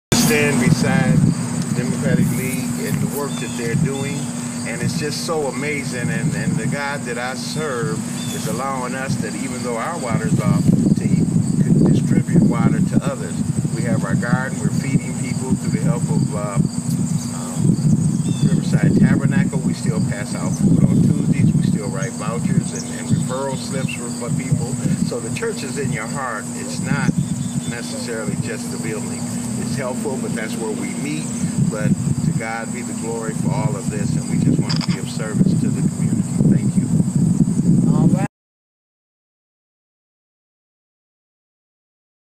Flint's Emergency Water Relief ~ Site #1 at Mission of Hope
The City of Flint has continued to charge unfair water rates to local residents while continuing their poor water quality service. The Mission of Hope is headed by Pastor Bobby Jackson who assists more than 100 homeless people per week who need to do their laundry, eat, shower, and use the restrooms. However, since the Mission of Hope has been billed for over $2600.00, far more than the Mission of Hope can afford, their water has been turned off by the City of Flint. All under the dictatorship of Emergency Manager Darnell Early.